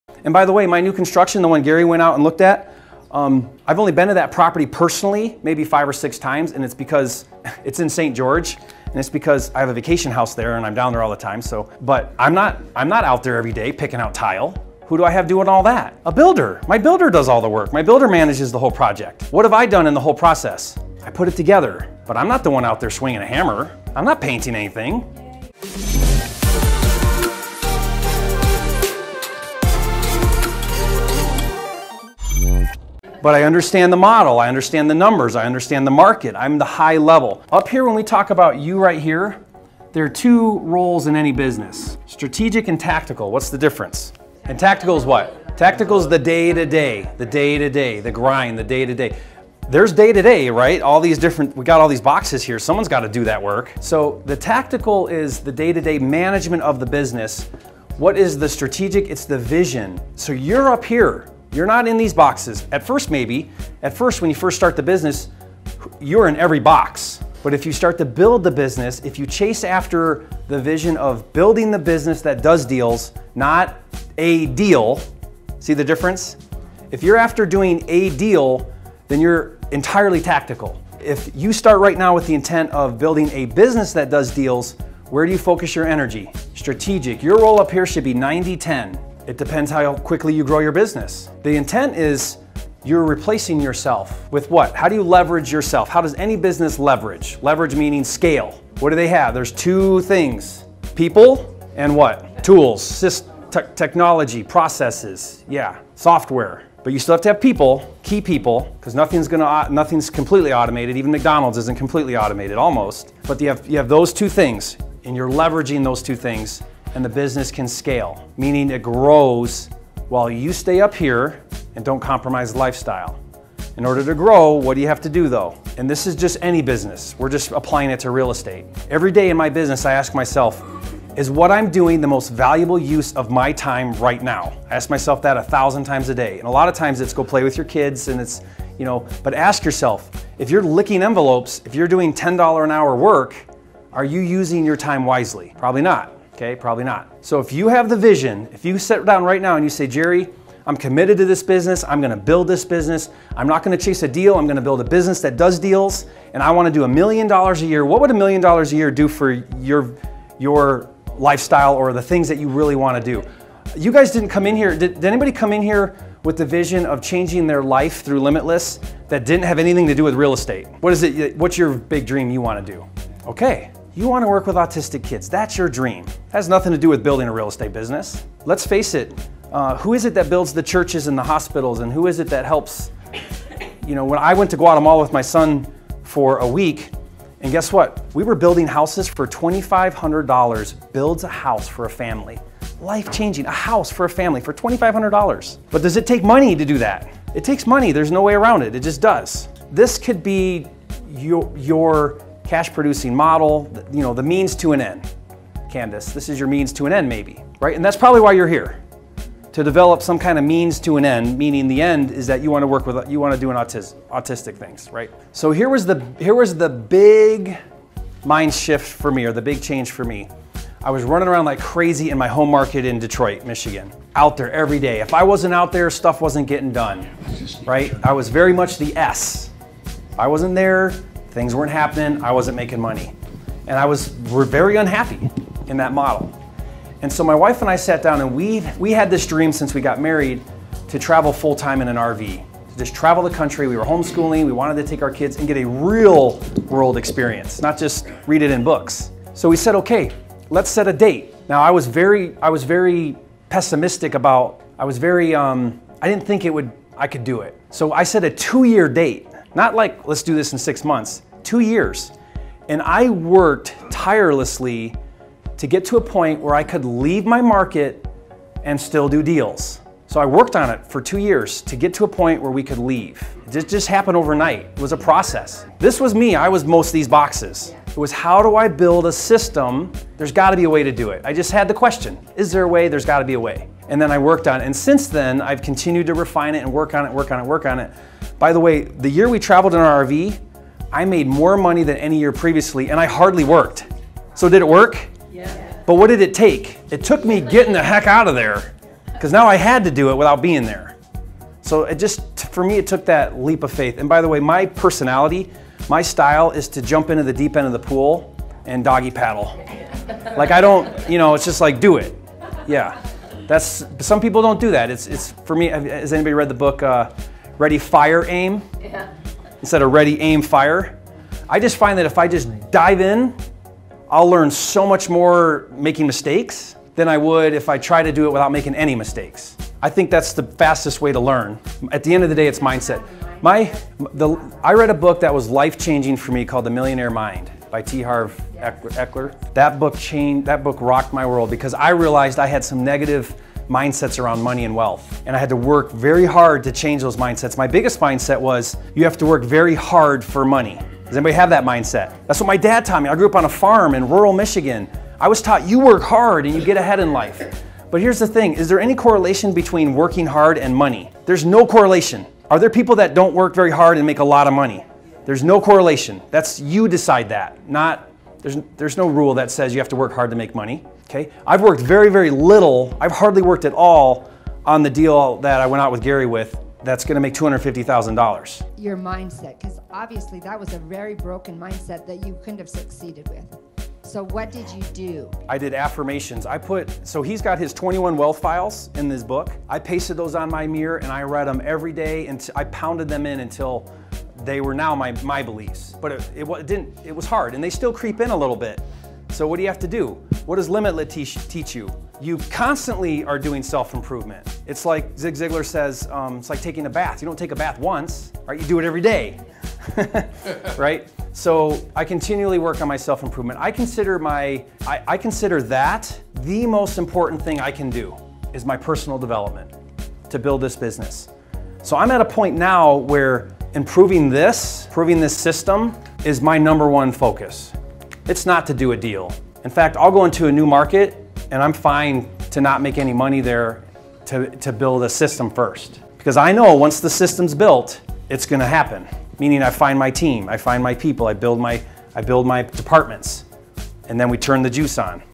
How To Grow A Real Estate Business - 2 Requirements
Scaling a real estate business requires that you spend 90% of your time, energy and effort doing strategic activities and only 10% doing tactical activities. On this video, learn how to grow a real estate business...

Jerry Norton is the nation's leading expert on flipping houses and has taught thousands of people how to live their dream lifestyle through real estate.